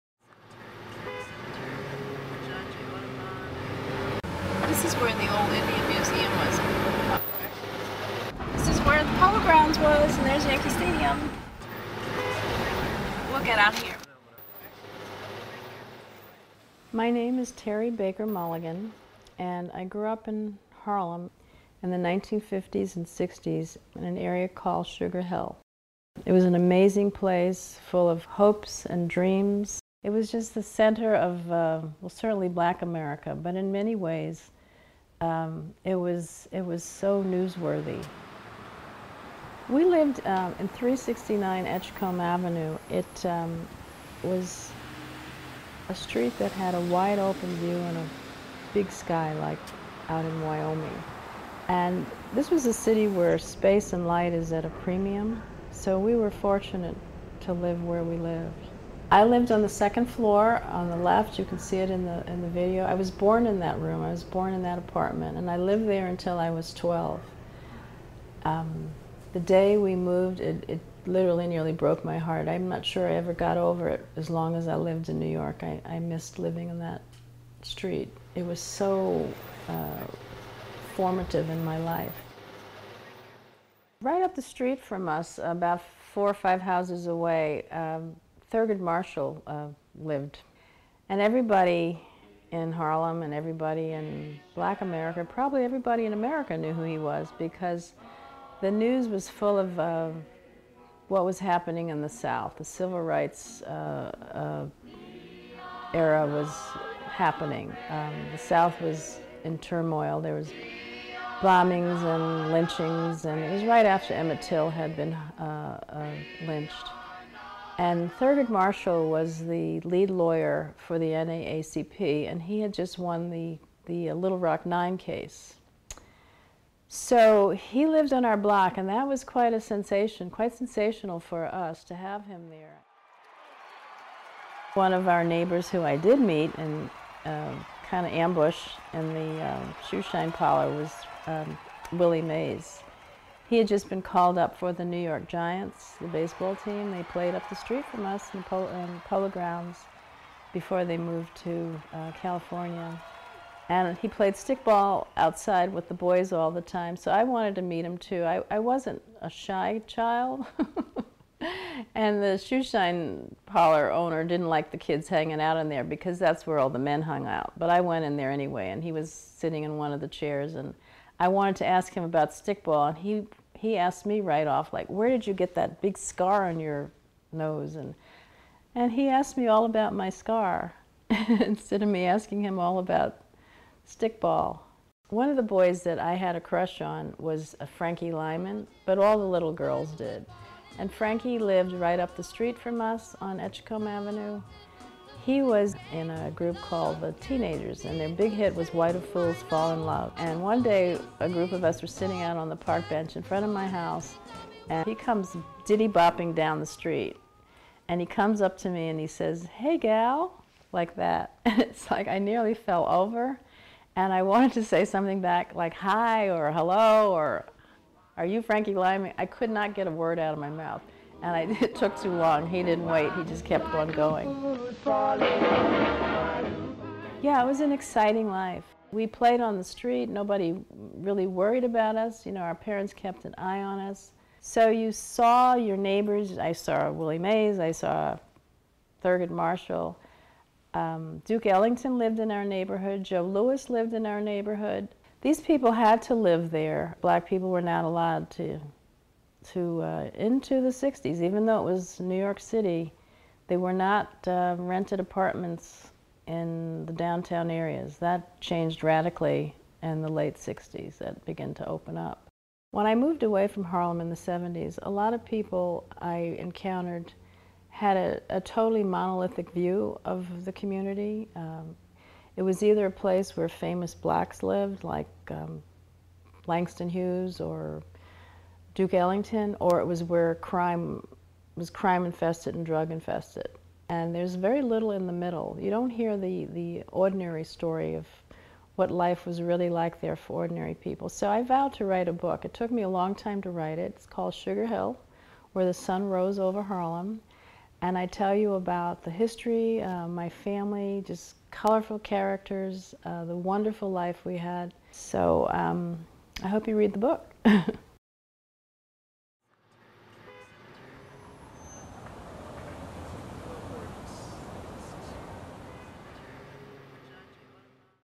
Sugar Hill, Where the Sun Rose Over Harlem
Terry Baker Mulligan describes her life growing up in Harlem in the 1950s and '60s, based on her memoir, "Sugar Hill."